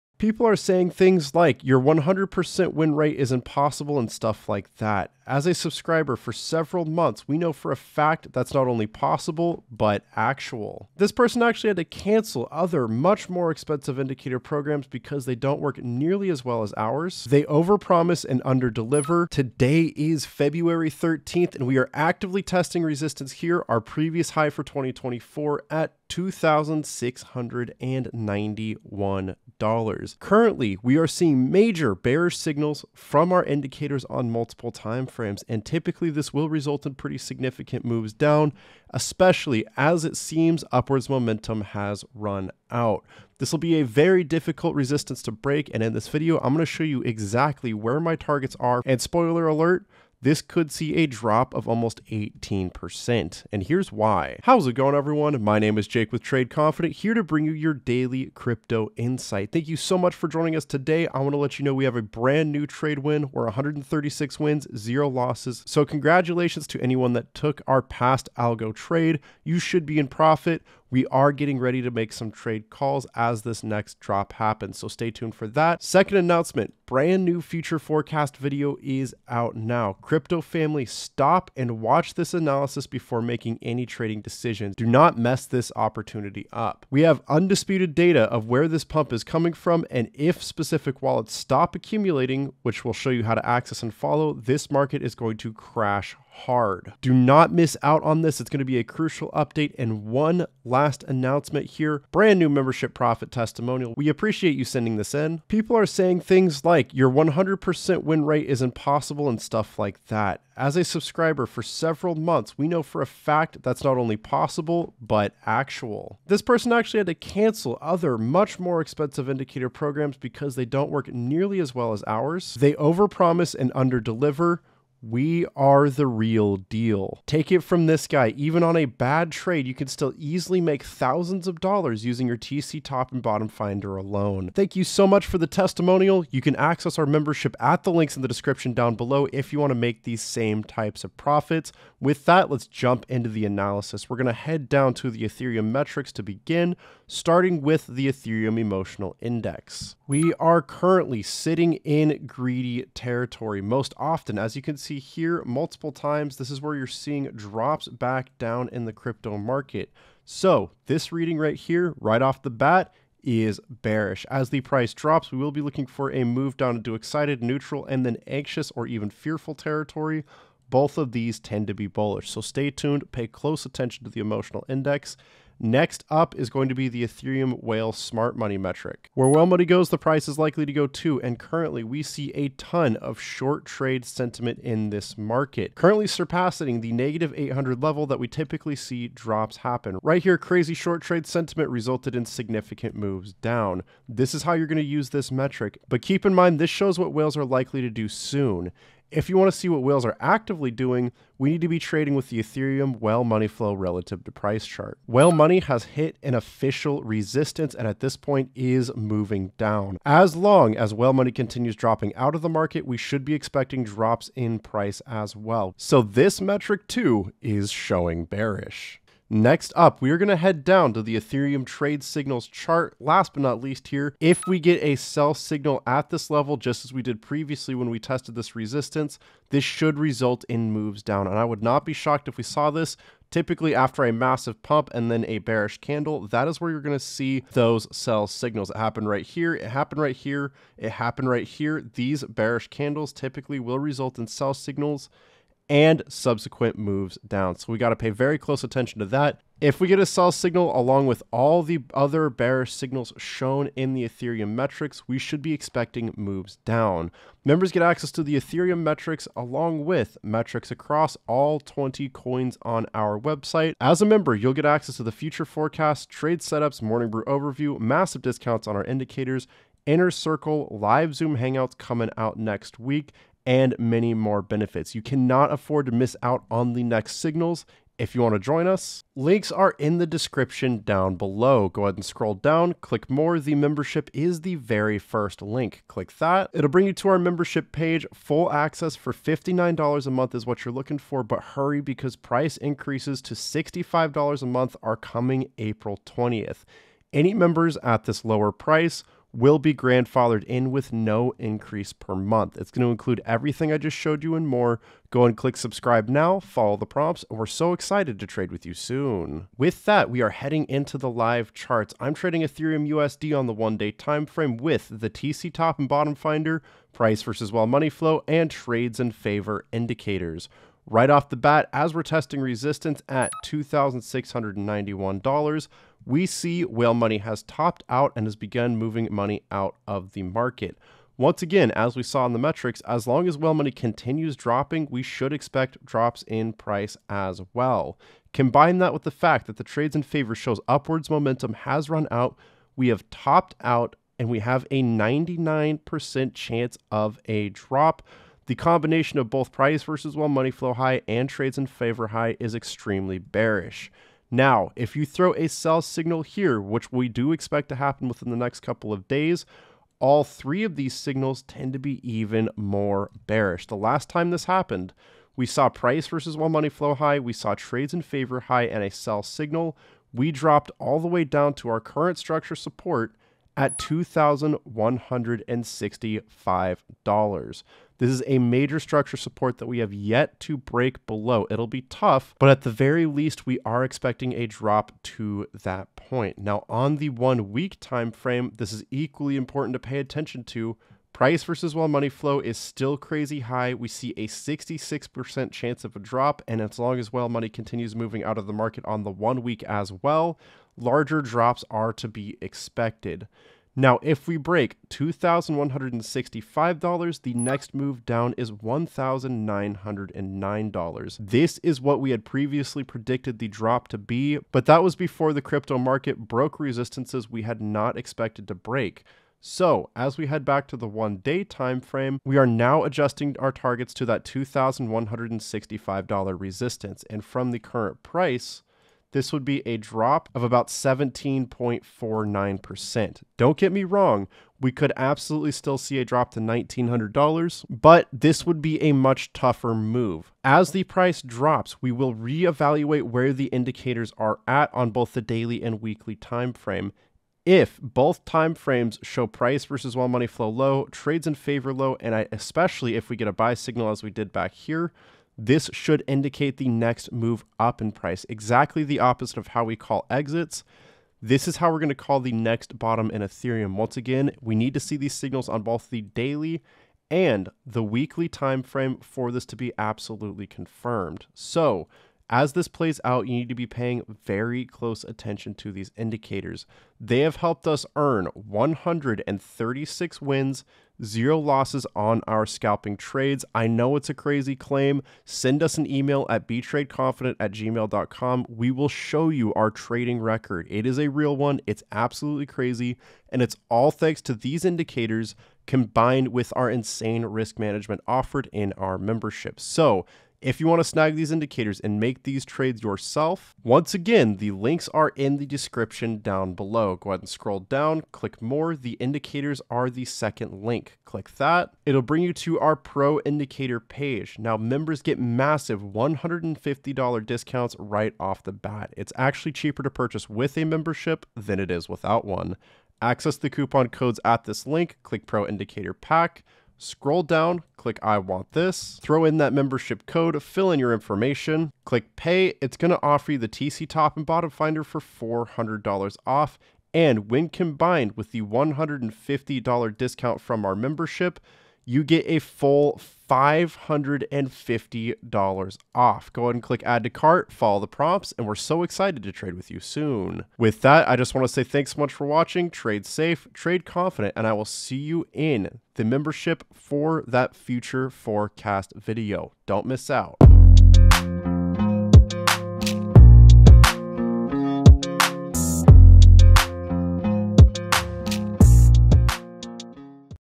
Ethereum Whales SELLING!! Are You? (-17.49% Crash Incoming?) | ETH Price Prediction
✅ Join Membership: 136 Wins/0 Losses and Join Our $1M Account Growth Journey

In today’s Ethereum price prediction video, I'll show you Ethereum traders MUST prepare for the next move. Whales are actively selling out of the market, and the last time we got our current combination of signals, Ethereum dropped SIGNIFICANTLY! With multiple data points showing bearish on multiple timeframes, just how far can we drop? Our analysis show s a possible -17.49% crash incoming. Ultimately, we are at the mercy of the whales. Here's what you need to know!

Intro: 0:19
Announcements: 0:54
Analysis: 2:21

Video Topics